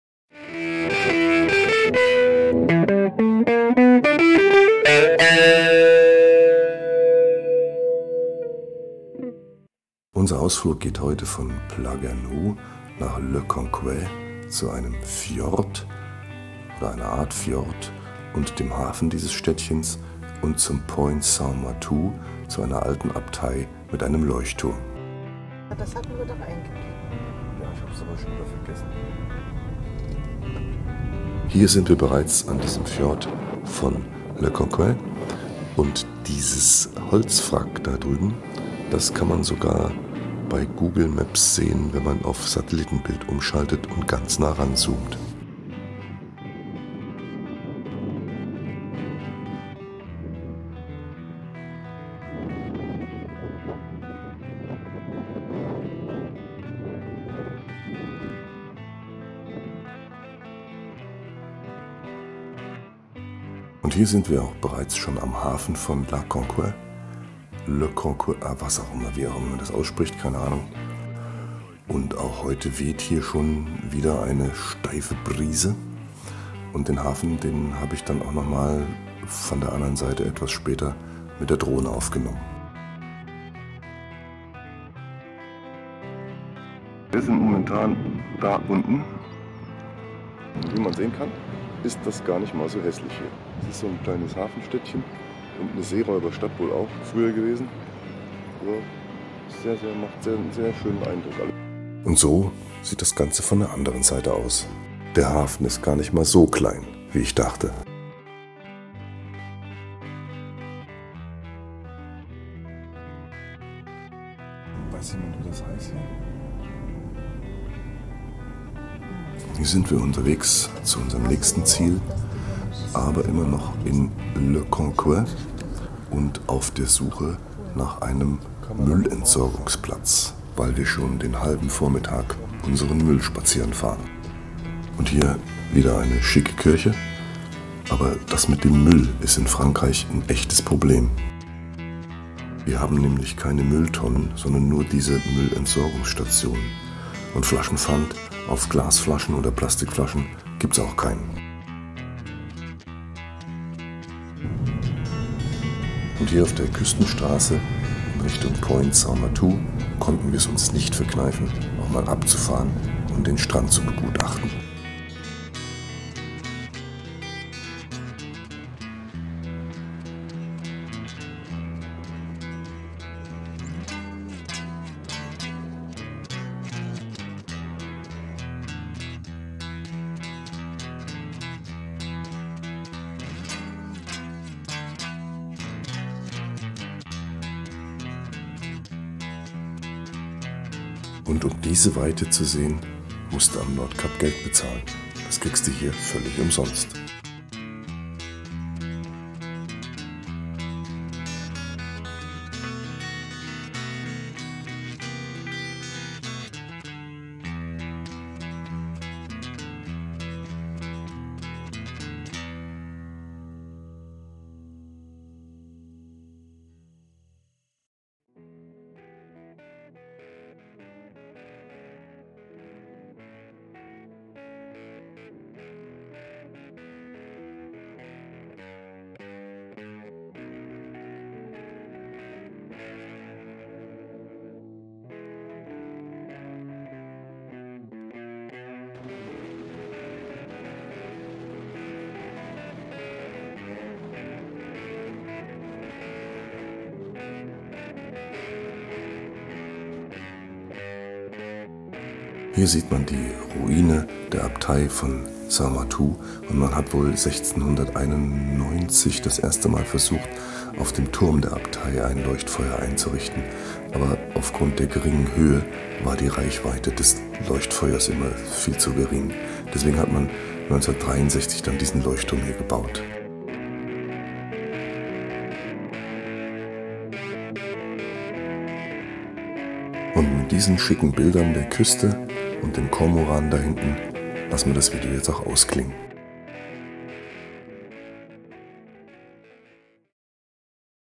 Normandie April 2019 - Müllensorgen in Frankreich. Flaschenpfand gleich Fehlanzeige.
Ein Ausflug an die Küste der Bretagne